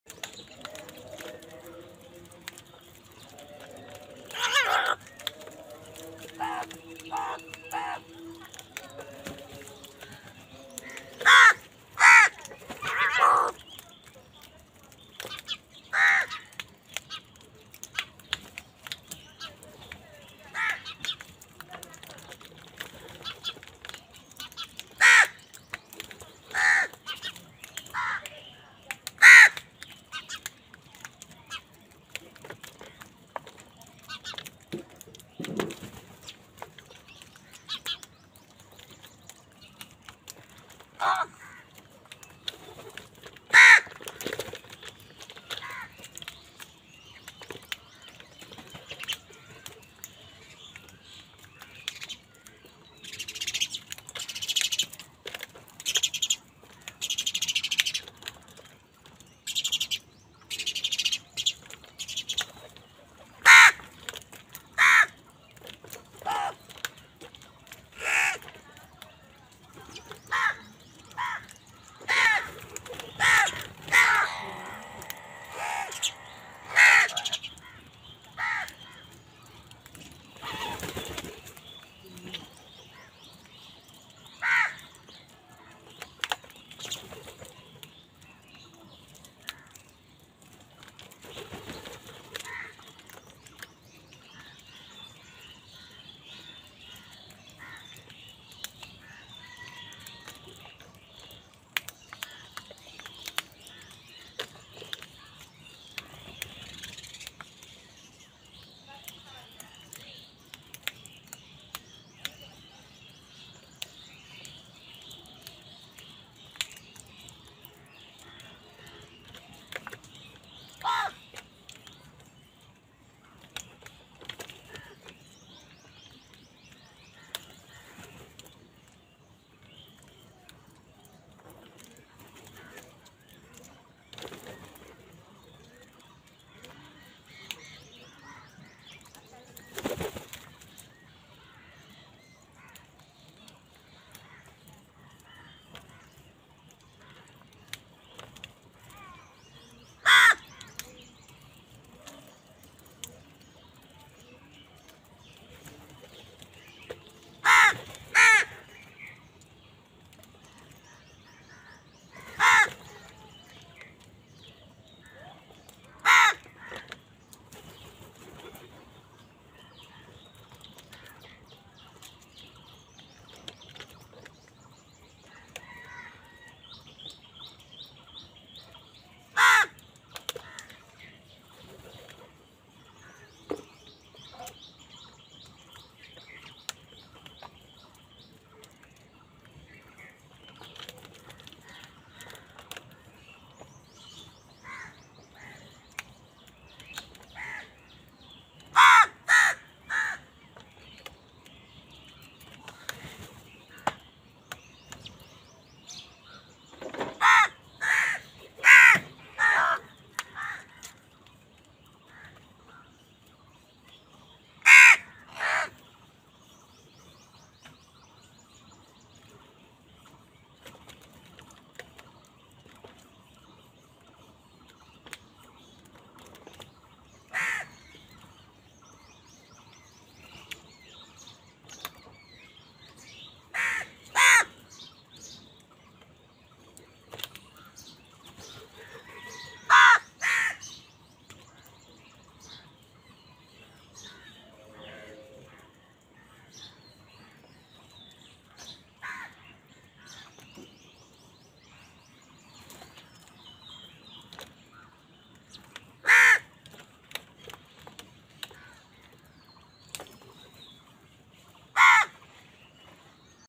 কাকের ডাক Crow Sound Crow ki Awaz Loudly part | Crow bird Fight for Food |Bird and Animal Soun452
কাকের ডাক Crow Sound Crow ki Awaz Loudly part  | Crow bird Fight for Food |Bird and Animal Sou452

So, Crow Sound part 71... Well, the video gives you more fun and you can find how to crow fight for food
Hey guys! hope you like this Crow Sound Video